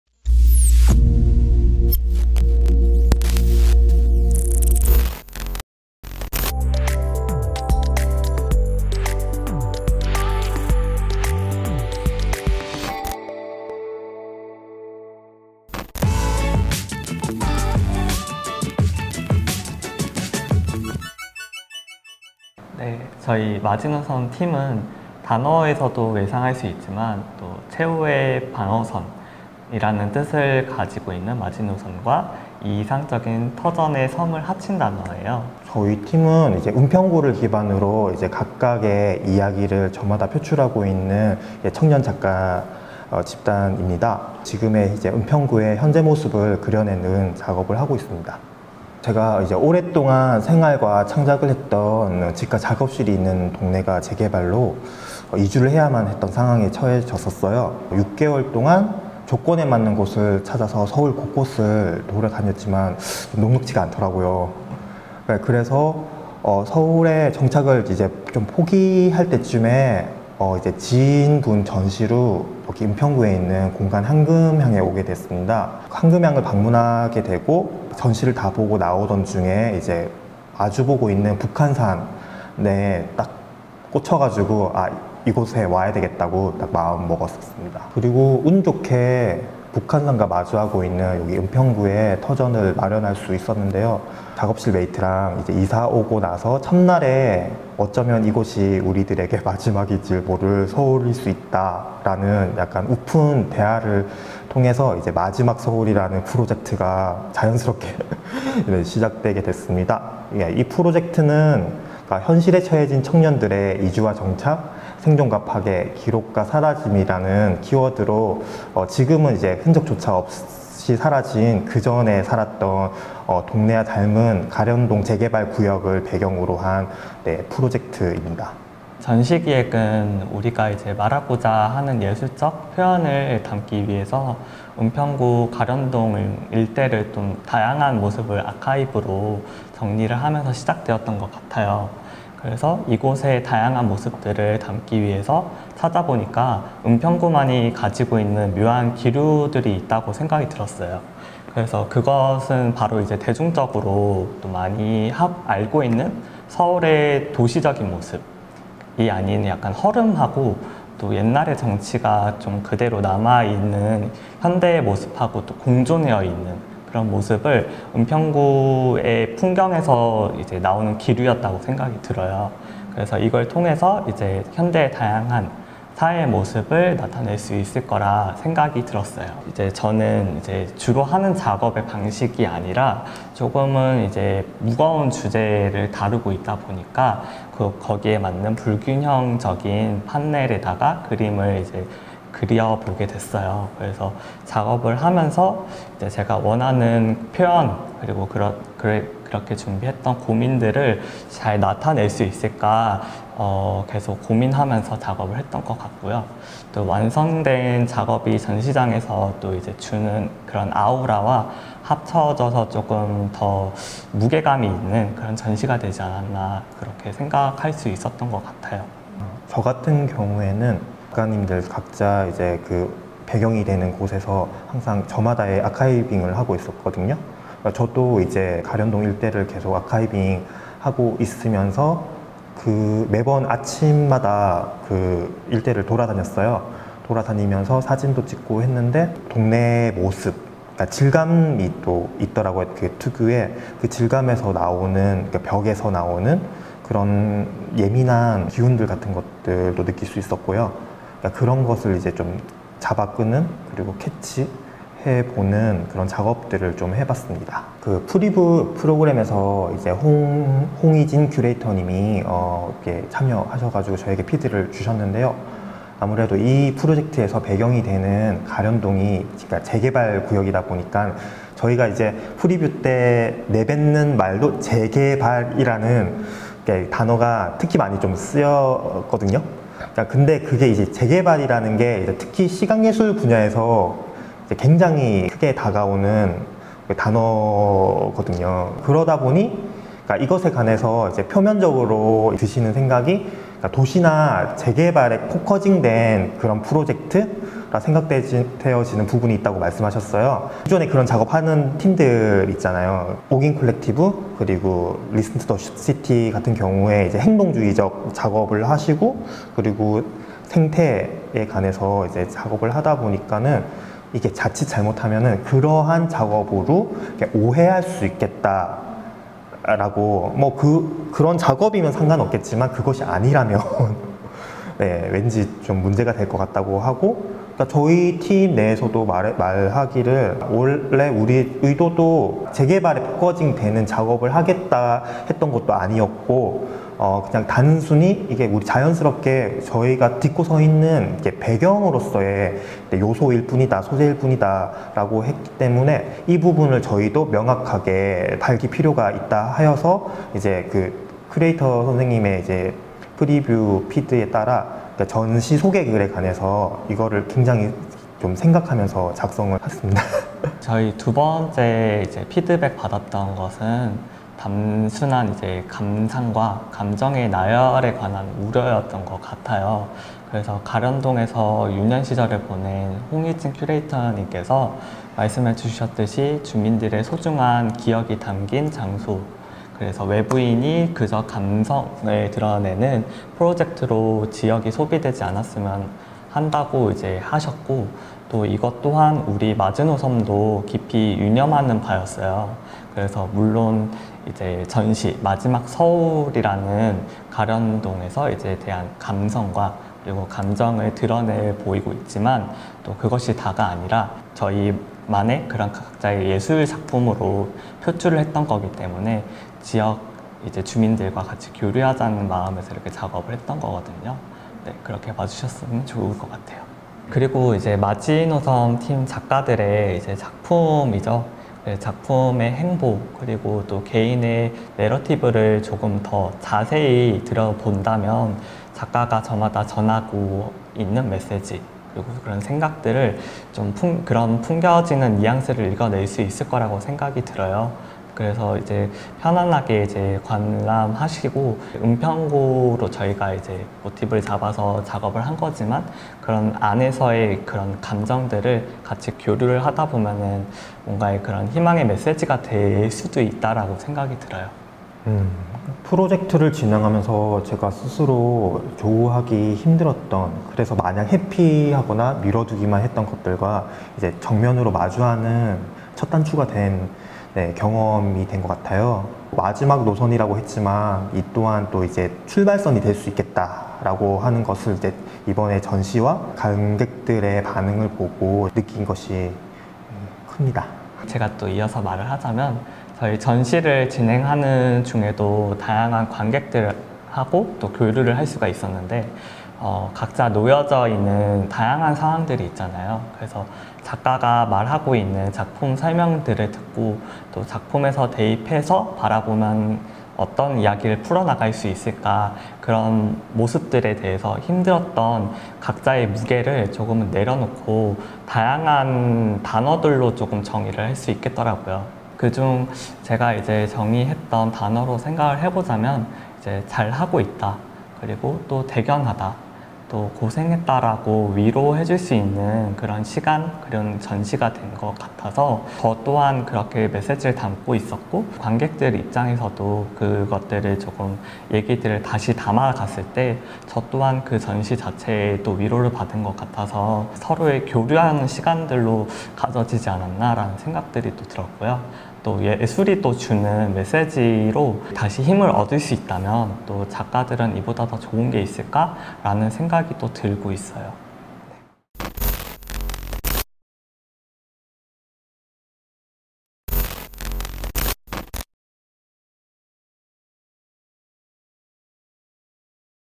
[은평 지역문화예술 위크 WEEK] 마지노섬(마지막 서울)_세션1. 은평 지역문화예술 콘텐츠 공유회_청년예술가 지역연계 지원
[은평 지역문화예술 위크 WEEK] 은평 지역문화예술 콘텐츠 공유회

청년예술 지역연계 지원

지진현 시각예술작가, 정영우 시각예술작가(마지노섬) - 마지막 서울
00:22 팀소개
00:48 프로젝트 소개
02:09 작업과정 소개
04:18 초반기획과 달라진 부분
07:35 프로젝트를 진행하면서 느낀 점

은평 지역문화예술 위크 WEEK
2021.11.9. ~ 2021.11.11
은평문화재단 유튜브, 인스타그램 온라인 개최

▼‘은평 지역문화예술 위크 WEEK’에 대해 더 알고 싶다면?▼